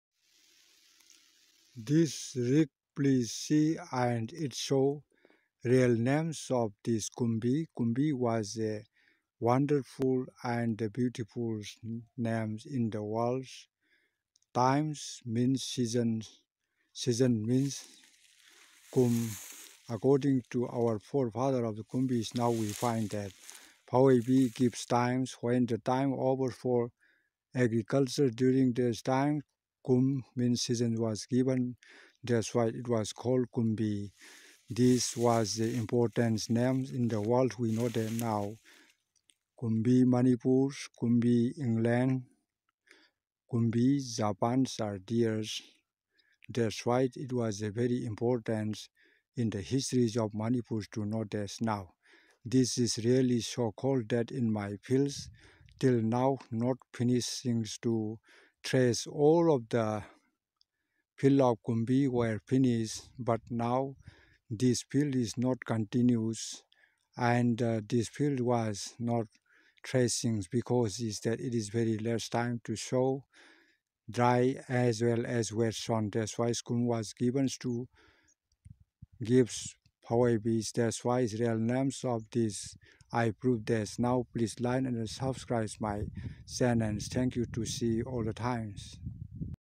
Experiment real name of kumbi, why was called it? "Rick,, ( phoubei) in Manipur language.
" Kumbi" Is famous name  in the world."kum,, means season, bi means given. Agriculture time was over in December last week 27december in 2021 butPhouoibi gave season for crops of rice in kumbi. So it was called Kumbi. Look this " Rick,, still harvesing is done not finished  on 22 December 2021.My croaping season is given me. It was written Mythologist by Houjam yairangchand Meithei, Manipur state, India🇮🇳.